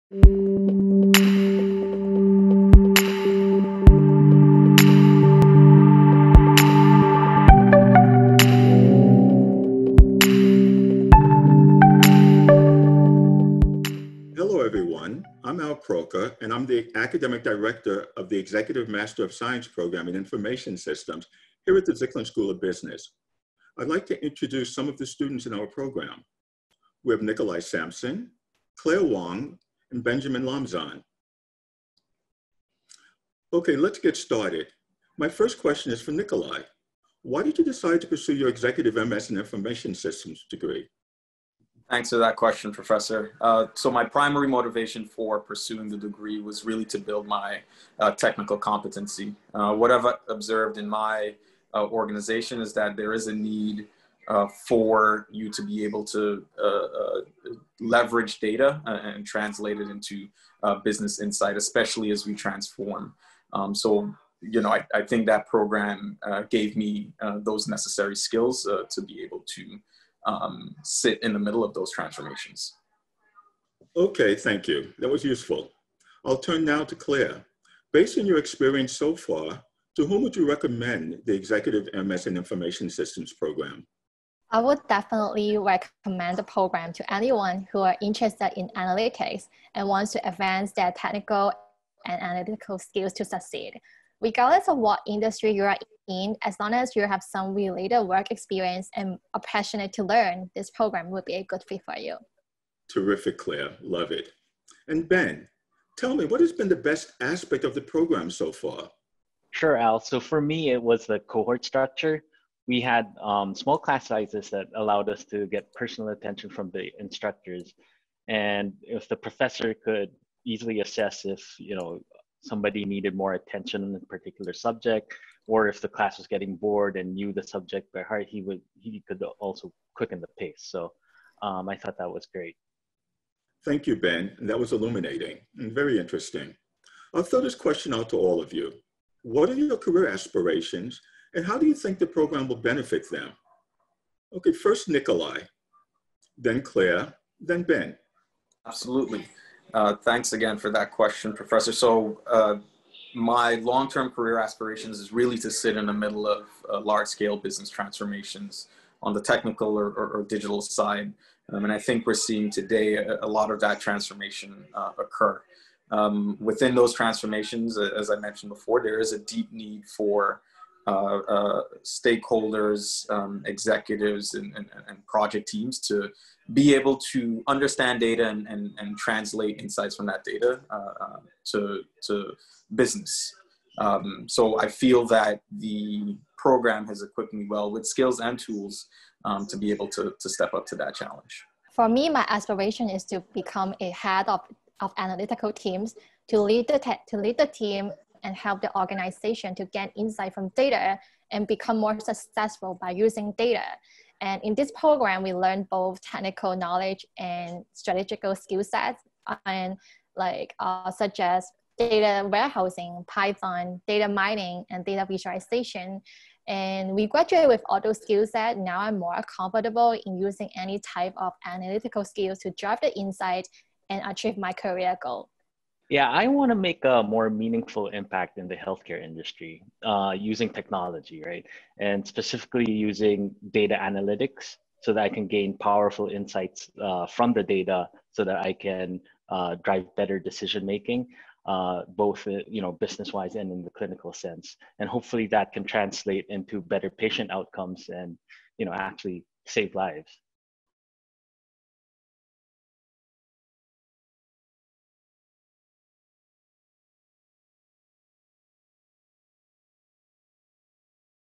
Zicklin's Executive MS in Information Systems
The Executive MS in IS with a concentration in Data Analytics is custom-designed for managers seeking the expertise to manage technology effectively and implement sound information systems strategies.

Skillful management of technology is critical to fostering innovation and providing a strategic edge for your organization. This technological imperative, combined with the exponential growth of data and sophisticated quantitative tools, means that executives across the business landscape need technical and analytical skills to succeed.

This program provides comprehensive coverage of both the technical knowledge and strategic skill set most needed by today’s executives. At the same time, it offers a deep focus on the tools and methods of data analytics that will give you the managerial skills necessary to successfully initiate, run, and evaluate full-scale data analytics projects.

The program covers a wide range of topics such as data warehousing, Python programming for data analytics, data mining, and data visualization. It will also provide you with hands-on experience using the latest software tools and methods of statistical analysis.

Regardless of your background or prior technical expertise, you will gain strategic skills critical to managing technological change now and in the future.

The Executive MS in Information Systems program leads to a Master of Science degree.